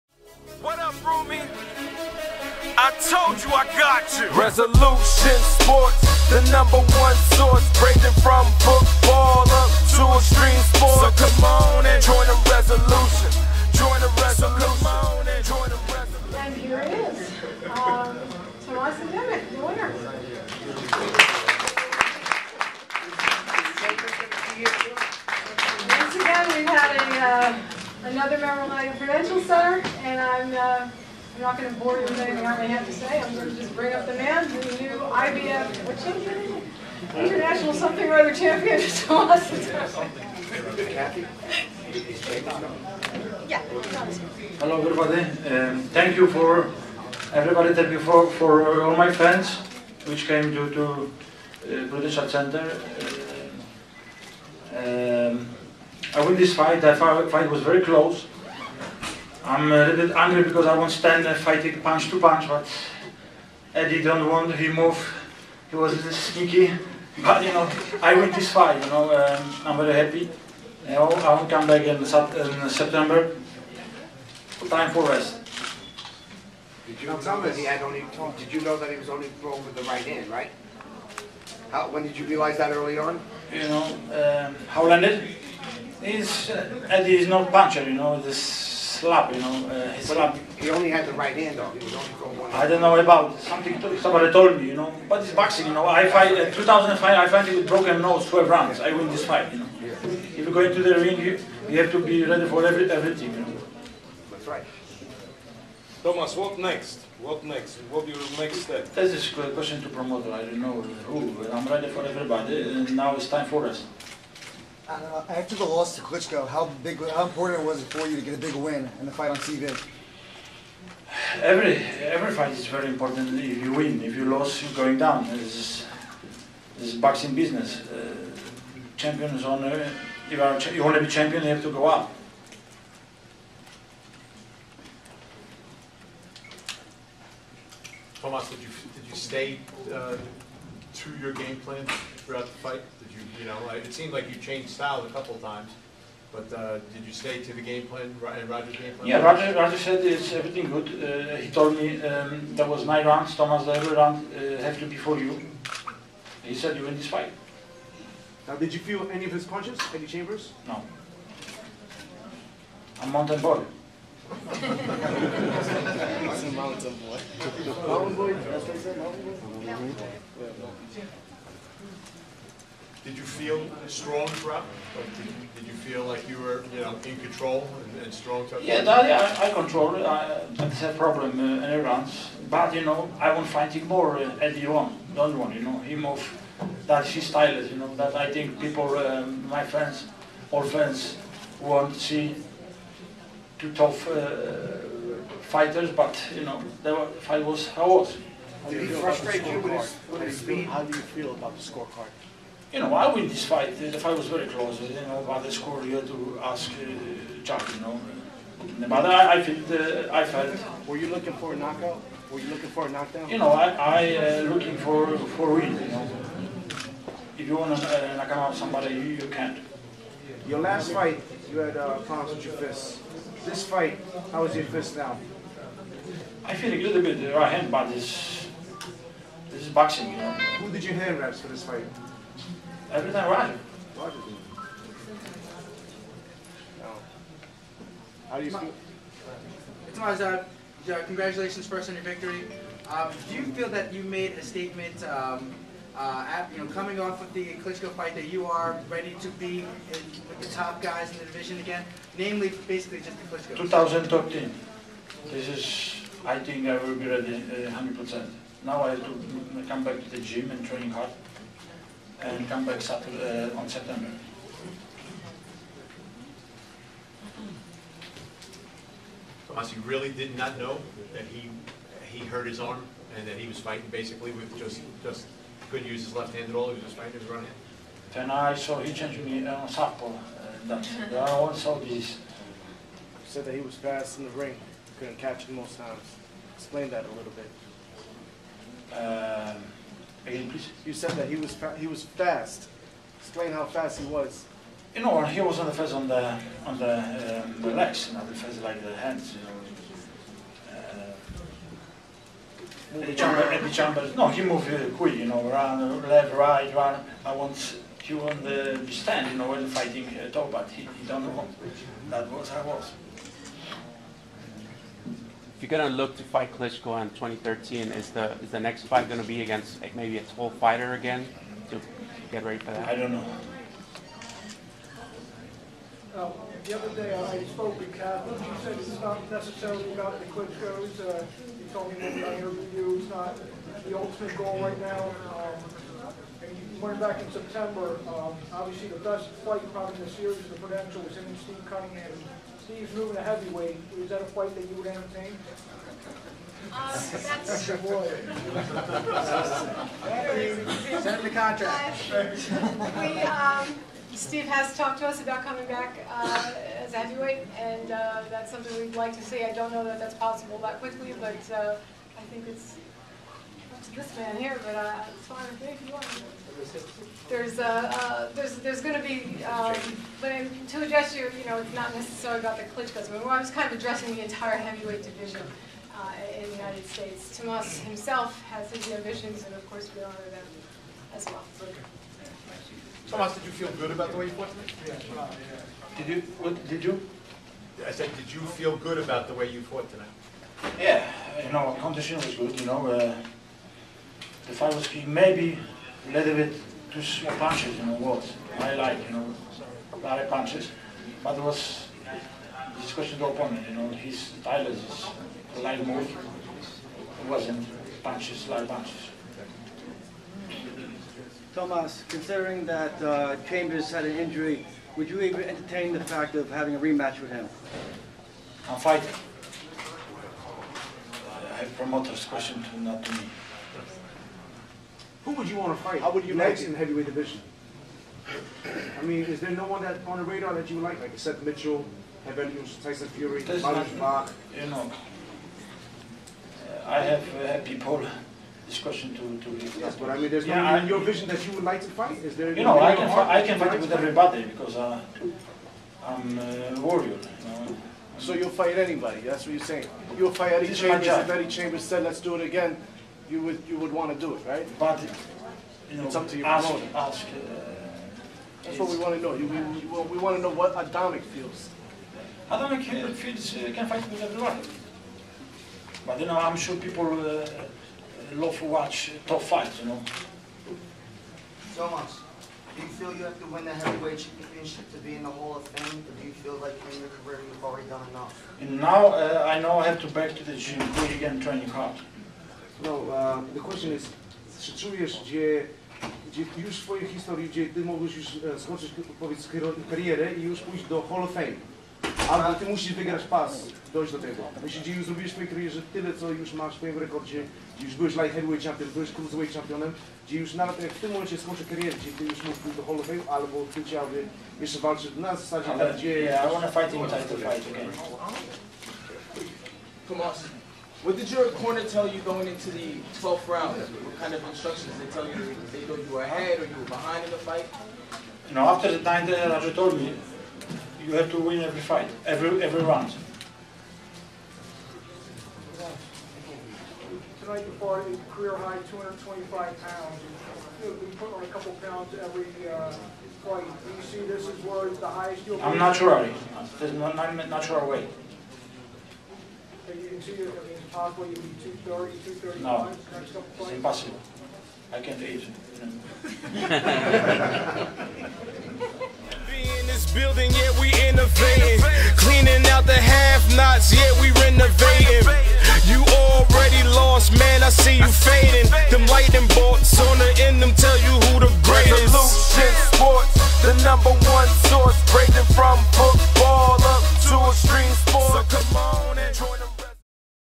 Tomasz Adamek Post-Fight | June 16, 2012
Heavyweight Tomasz Adamek speaks to the media at the post-fight press conference following his IBF North American Championship fight with Eddie Chambers at Prudential Center on the NBC Sports Network.